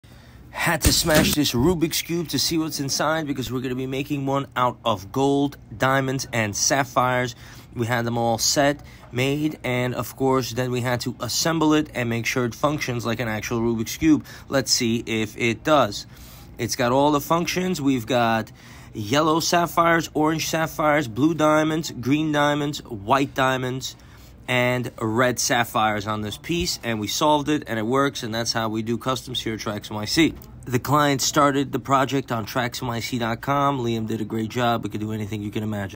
MAKING RUBIKS CUBE WITH GOLD & SAPPHIRES, & DIAMONDS!!!!
@Liam_trax had a customer reach out saying they wanted us to make them a custom Rubik's cube with gold, diamonds, and sapphires that would function just like a real Rubik's cube as well. Here's how we got it done, and how it works, of course, we had to staff solve the cube before the customer picked it up.